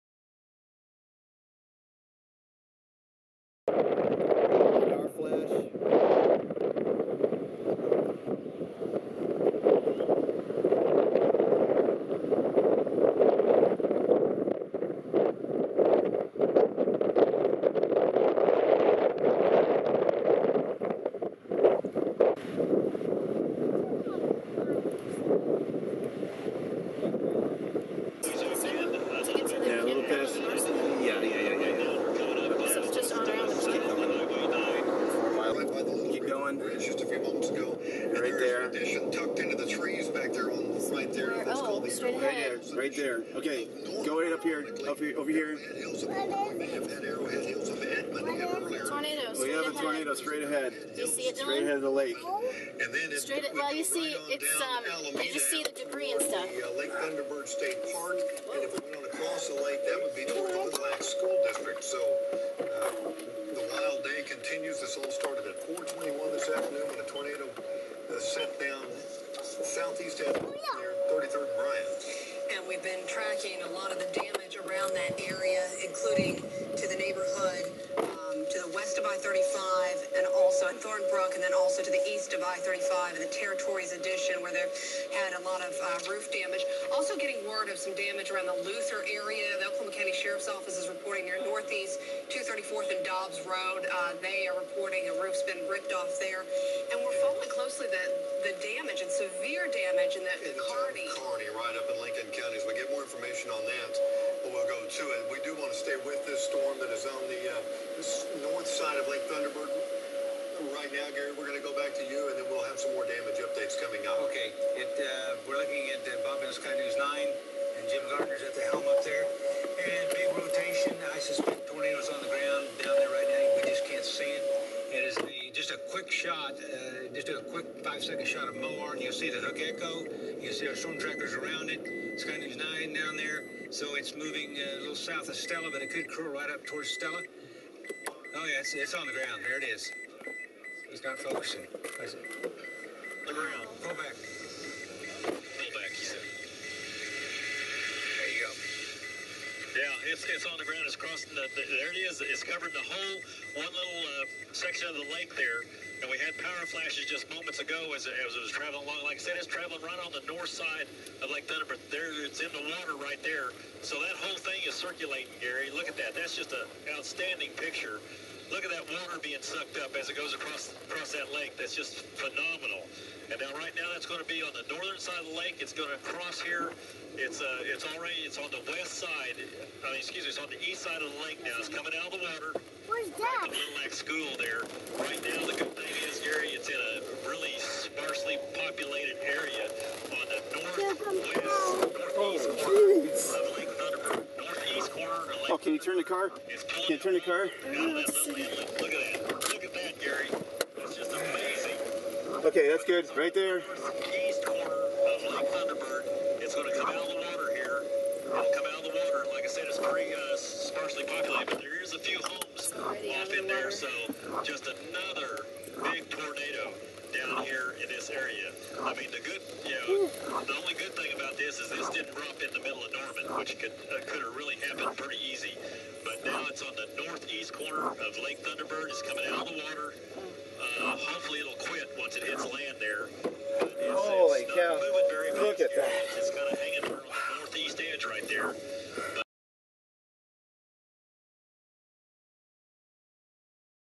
2013-May-19 Oklahoma tornadoes
The first part of the video is from the wedge tornado west of Wellston, OK.  This tornado had horizontal funnels, violent motion, and a huge debris ball on radar.  The second scene is from the onset of the east Norman to Shawnee, OK tornado as it crossed the north arm of Lake Thunderbird.